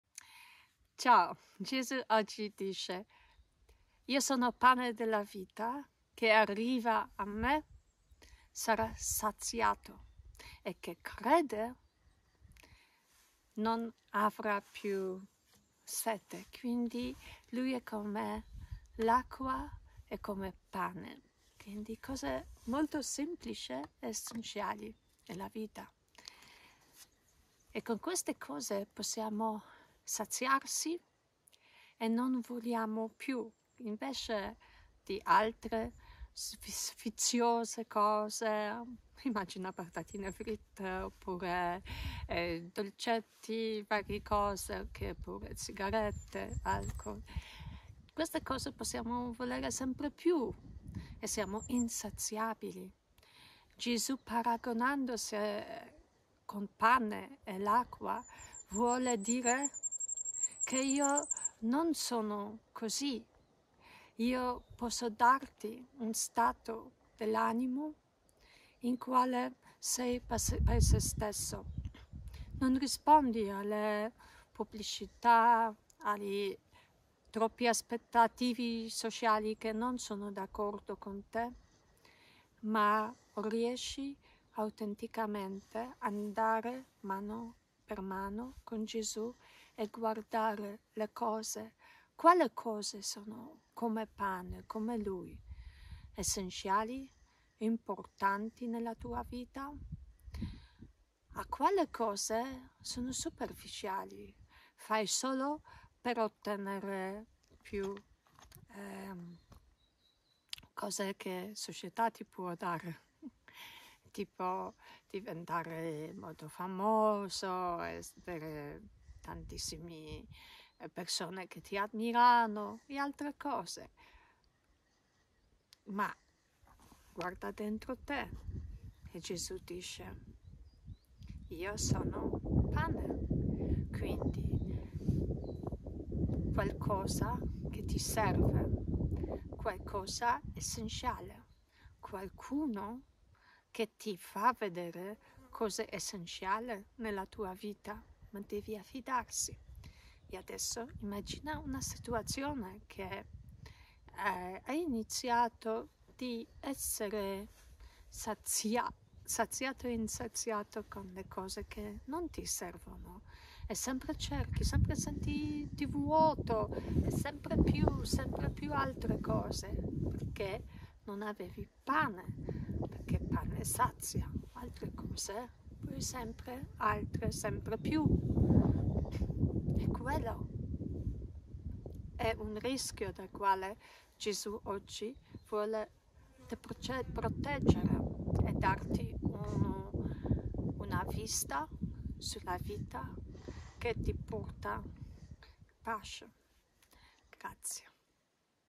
#116 Il pane della vita
#116 Il pane della vita (Gv 6,35-40): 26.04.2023
Il fatto che Dio si nasconda dietro questi semplici simboli come il pane e l'acqua/vino (nella odierna Evangelizzazione, si parla di sazietà e di sete) comporta il rischio di non riconoscerli. È più facile seguire ciò che non è fondamentale per la nostra vita e continuare ad avere fame e sete. Abbiamo il coraggio di vivere queste questioni fondamentali, anche se sono così "insignificanti"?